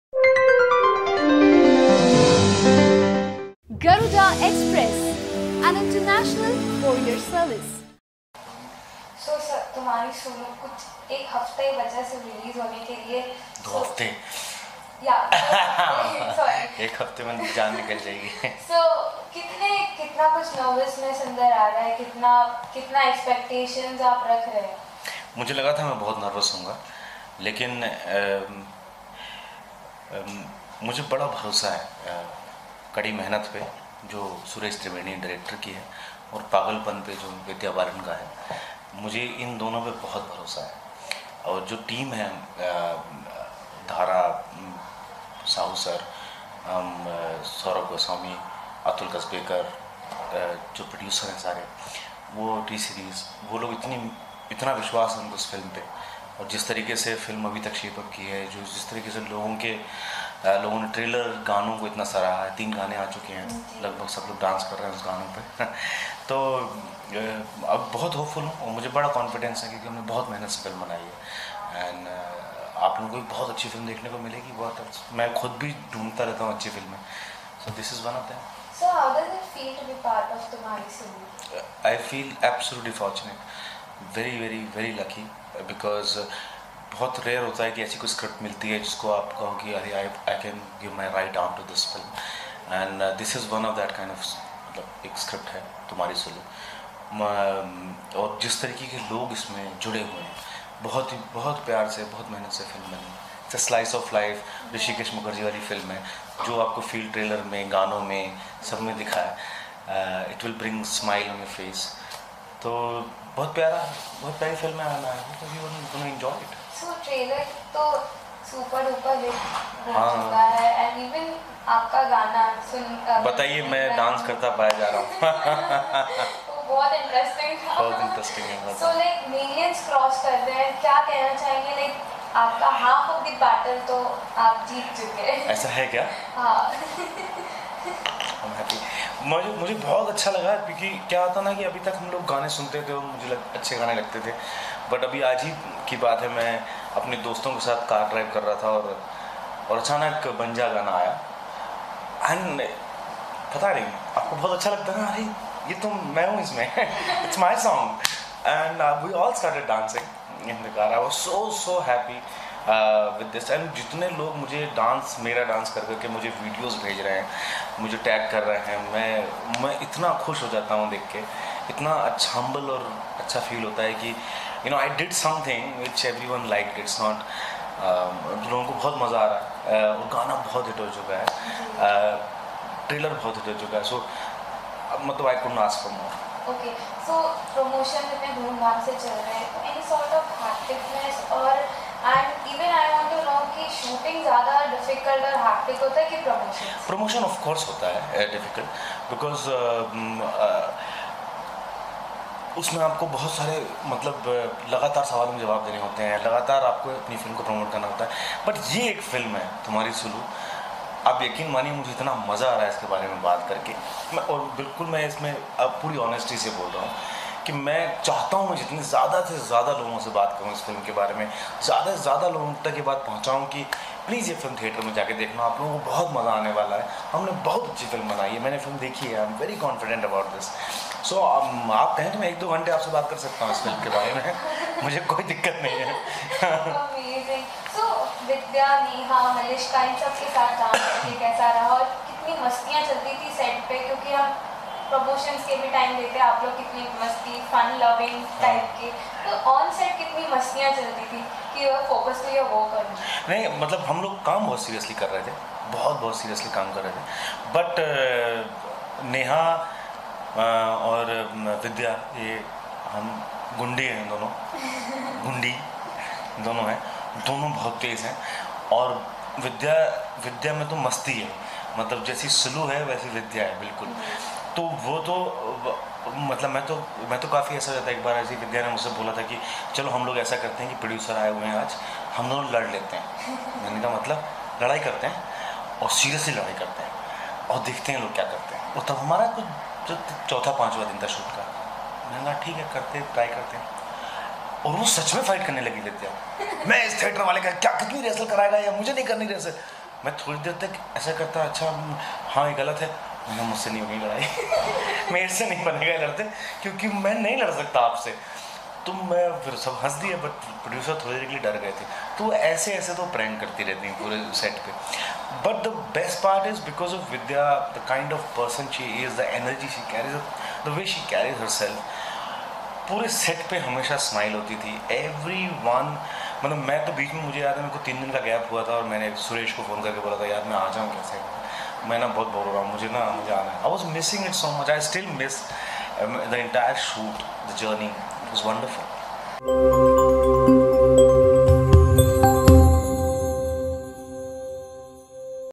Interview of Manav Kaul for the film ‘Tumhari Sulu’ | Vidyabalan | YOYO Cine Talkies
Interview of Manav Kaul for the film ‘Tumhari Sulu.
Vidyabalan